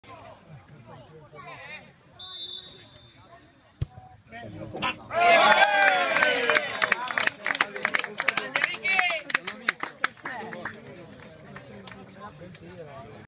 Coppa Italia Eccellenza: Olmo-Fossano 1-2, la punizione di Romani
La bellissima punizione del giocatore del Fossano, che scavalcando la barriera ha segnato il 2-1